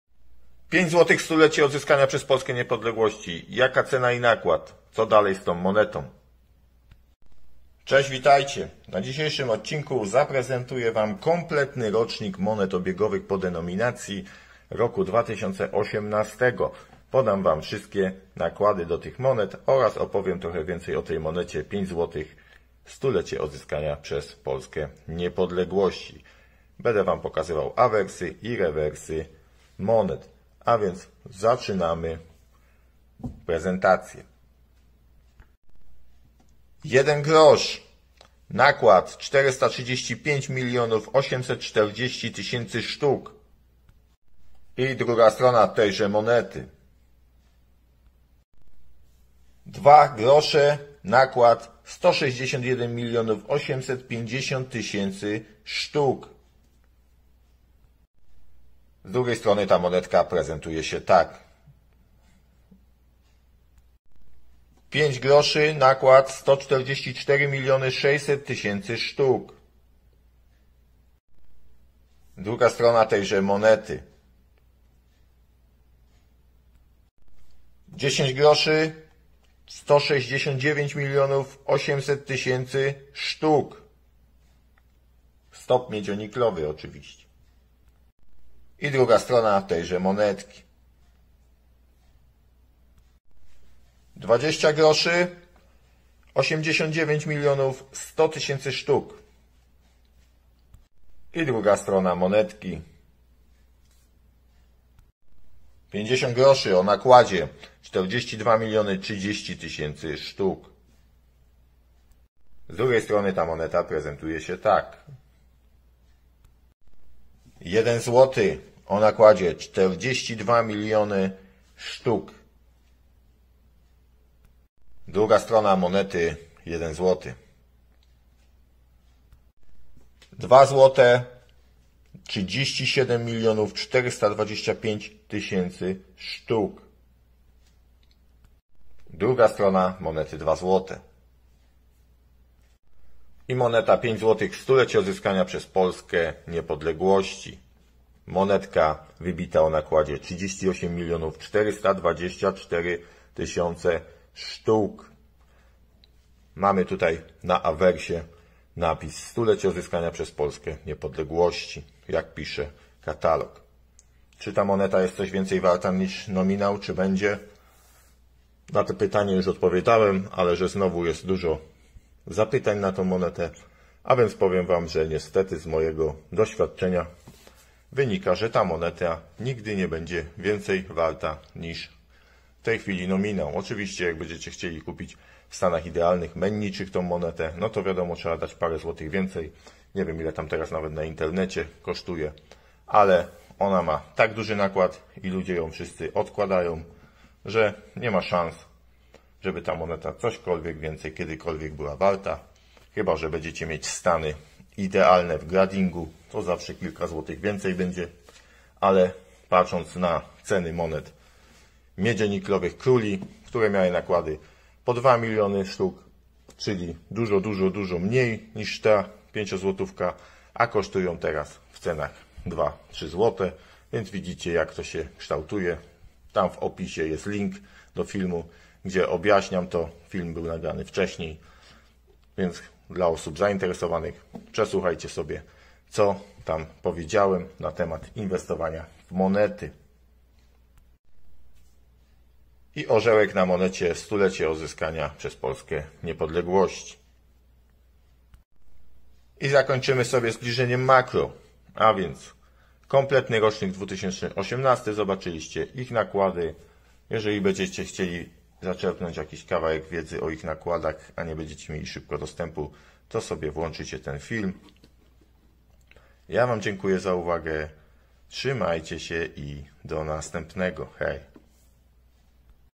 Komplet 2018 oraz nakłady, 5 złotych 100 lecie odzyskania przez Polskę niepodległości oraz jej cena.
Witajcie, dzisiaj podaje i prezentuje monety z roku 2018 ich nakłady oraz monetę z tego rocznika 5 złotych 100 lecie odzyskania przez Polskę niepodległości, 5złotych 100lecie #2018

Konkurs komunikujemy się pod najstarszym filmem.